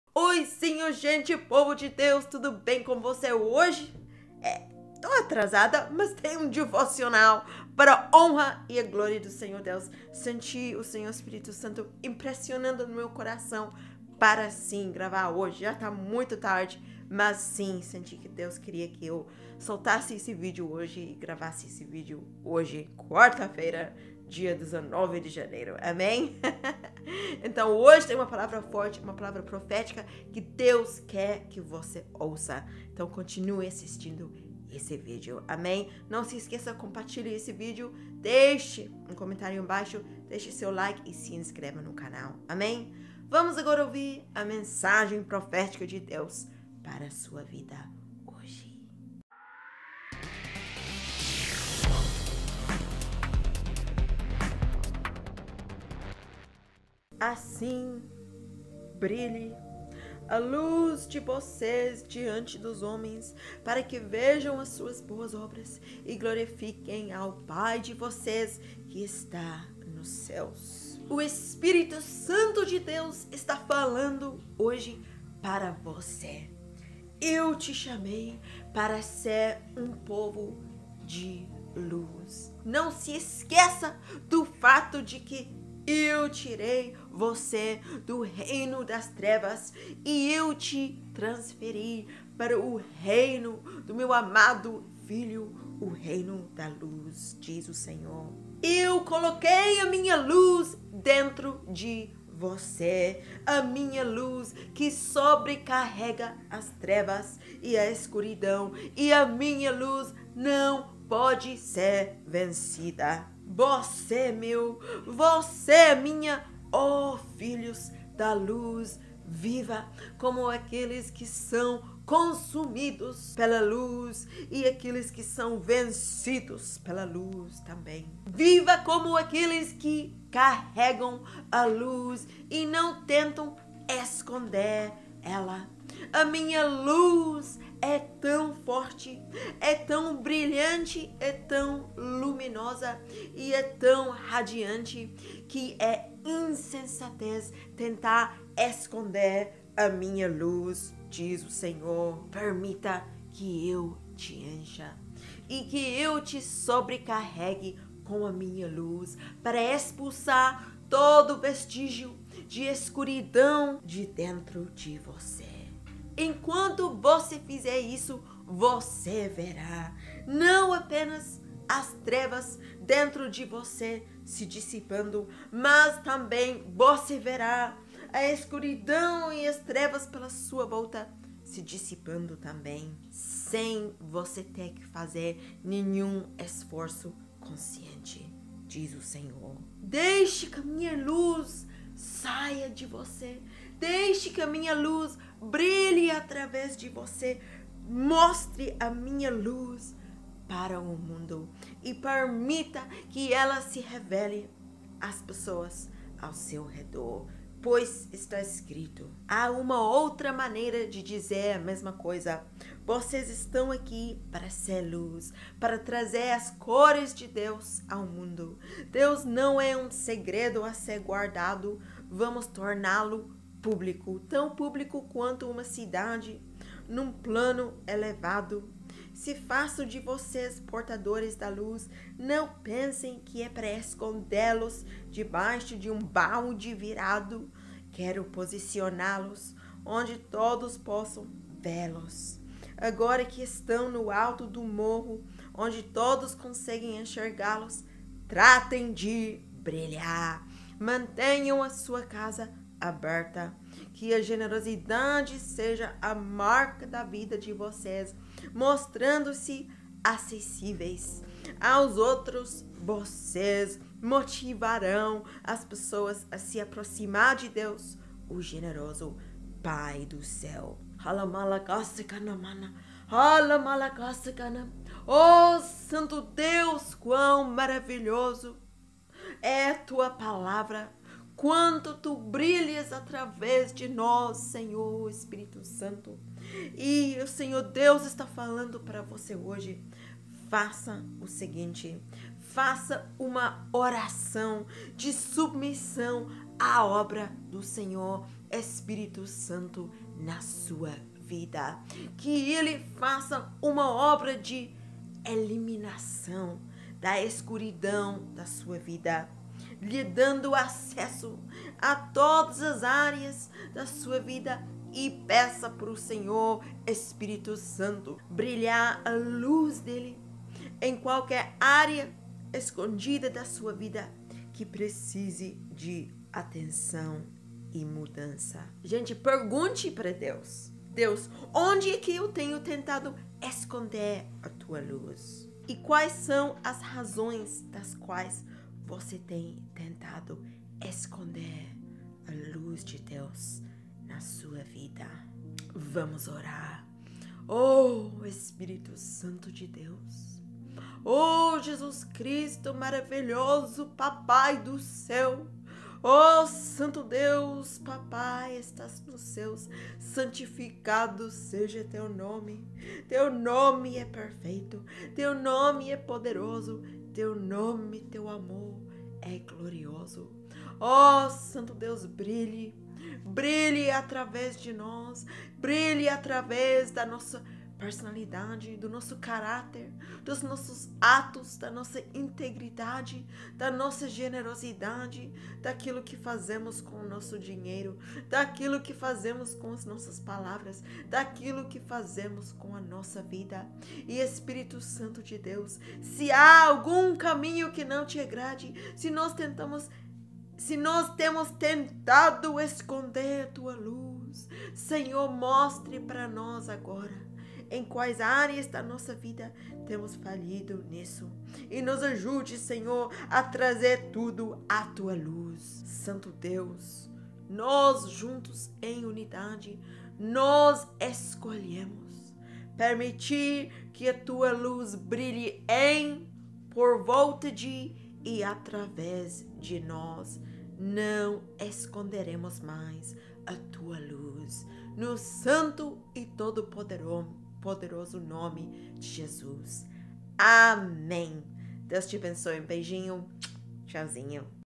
MENSAGEM PROFÉTICA DE DEUS SOBRE VOCÊ E A SUA LUZ 💡| Mensagem de Deus para hoje
“Há uma outra maneira de dizer a mesma coisa: vocês estão aqui para ser luz, para trazer as cores de Deus ao mundo. Deus não é um segredo a ser guardado. Vamos torná-lo público, tão público quanto uma cidade num plano elevado. Se faço de vocês portadores da luz, não pensem que é para escondê-los debaixo de um balde virado. Quero posicioná-los onde todos possam vê-los. Agora que estão no alto do morro, onde todos conseguem enxergá-los, tratem de brilhar! Mantenham sua casa aberta. Que a generosidade seja a marca da vida de vocês. Mostrando-se acessíveis aos outros, vocês motivarão as pessoas a se aproximar de Deus, o generoso Pai do céu”.

Assim brilhe a luz de vocês diante dos homens, para que vejam as suas boas obras e glorifiquem ao Pai de vocês, que está nos céus.”
‭‭Mateus‬ ‭5:16‬ ‭NVI‬‬

Também tem live da santa ceia as 20H todo primeiro sábado do mês. Marque aí na sua agenda!